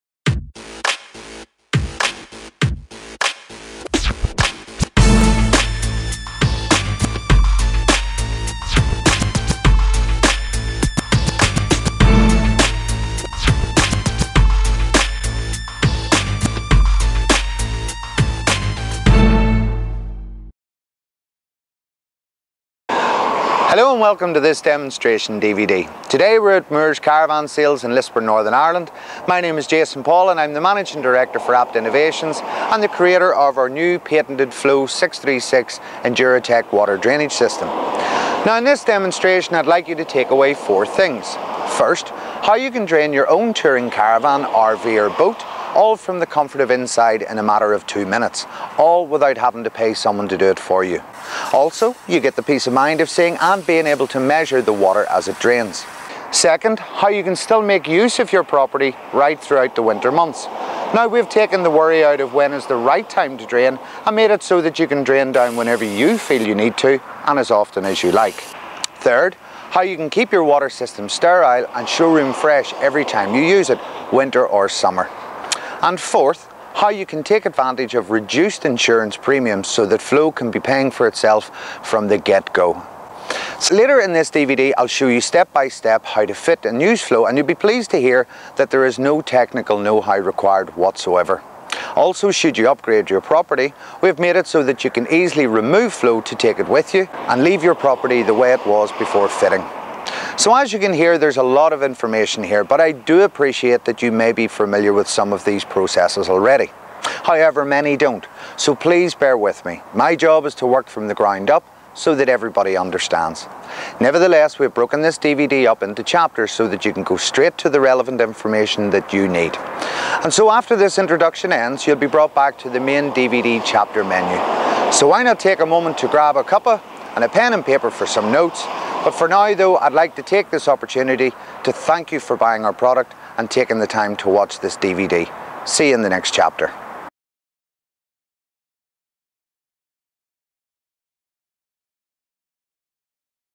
Floe Induratec 636 - Water Drainage System - Introduction
Drain down and winterise Touring Caravans, Motorhomes, Camper and Boats with the new Floë Integrated Water drainage system.

636 is the newest offering from APT Innovations Ltd. It is suitable for draining down and winterizing in approximately 2 minutes. Because it's so quick and convenient, this enables you to drain your own property, when and as often as you like, taking the worry out of when to drain.

Also, you'll never have to pay anyone to do it for you, while allowing you the benefit of using your property throughout the winter months, draining quickly when you leave. In addition, Floë will clean the inside of the pipes of bacterial buildup to 99.95%.

Floë is easily fitted to the property, but can be easily removed again, should you change your property.

To drain down with Floe 636, the only thing the owner needs to do is, drain the water heater, switch Floë on and then start opening and draining taps (faucets), showers and toilets. Everything is now operated from the inside the property, comfortably in the warmth.

With its own PCB brain and clever compressor, Floe will automatically stop at the correct pressure and restart automatically after each outlet is drained.

Thanks